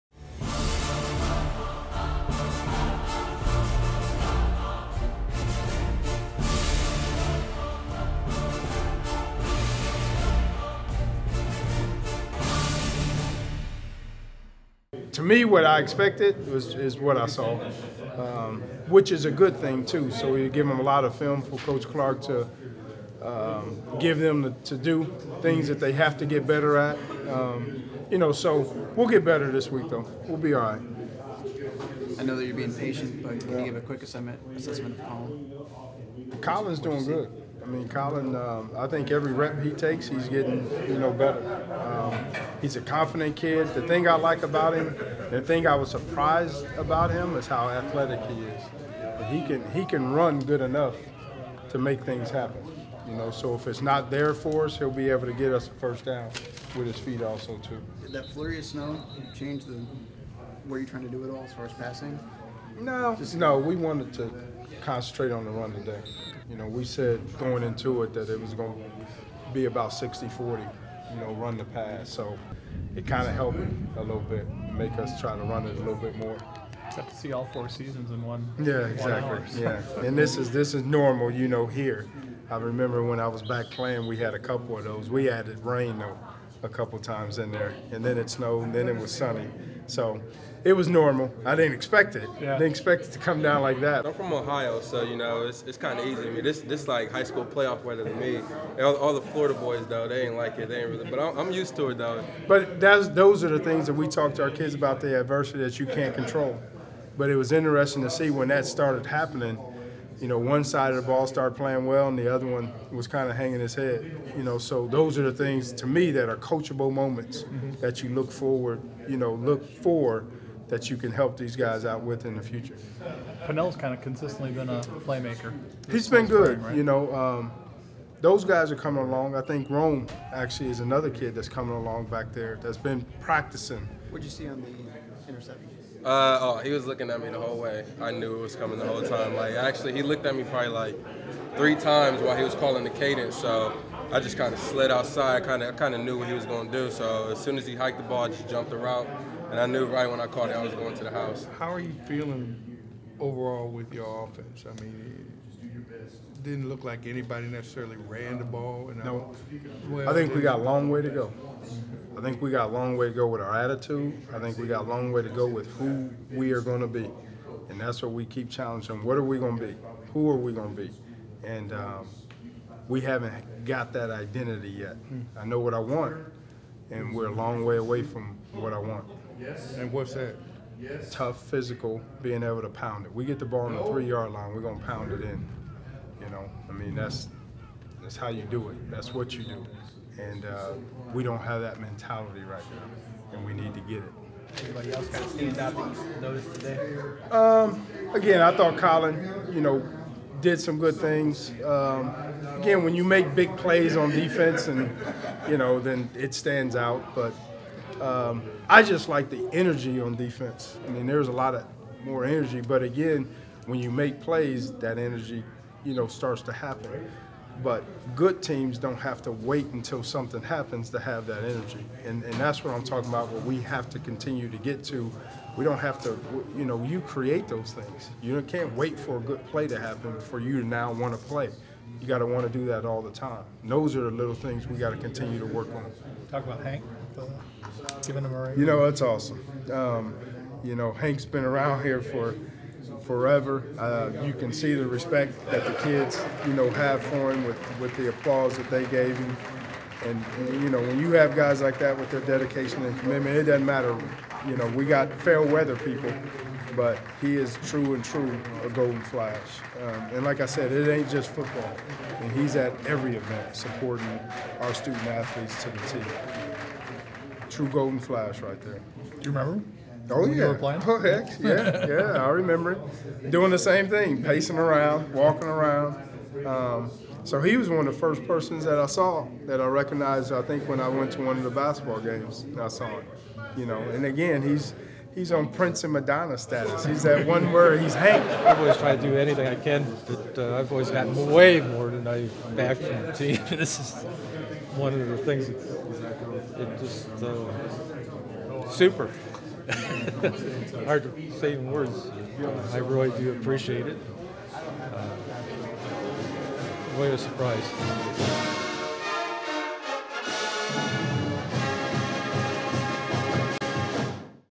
Kent State Football Jersey Scrimmage 4/20/13
Coach Haynes discusses what he saw in Saturday's Jersey Scrimmage and shares his thoughts on one of Kent State's biggest fans.